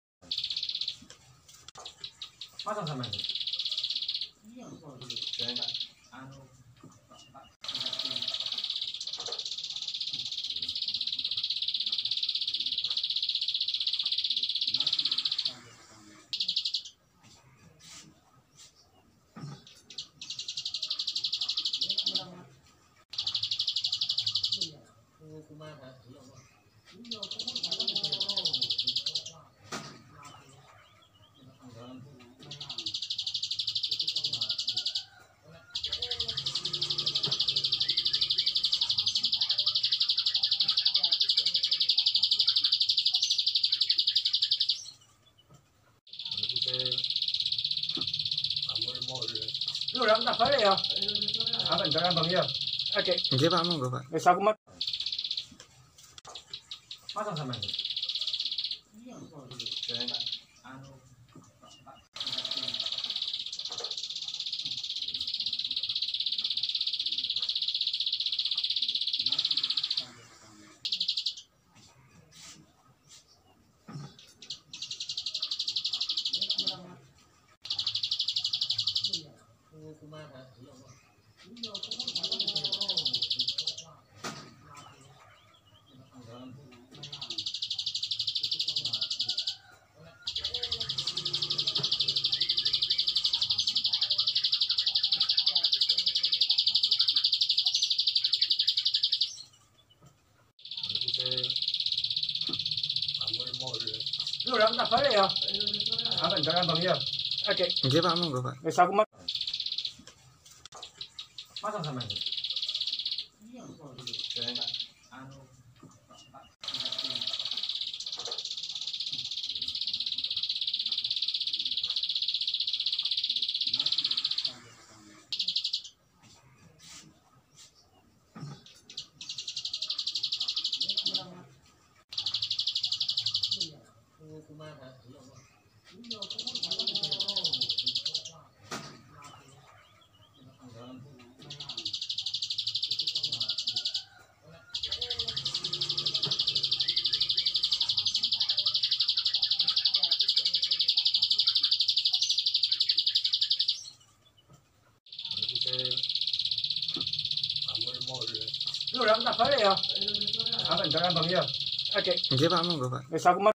Kicau Prenjak Alang Alang ngebren panjang ngeriiii
prenjak Alang Alang bisa jadi masteran alternatif yg luar biasa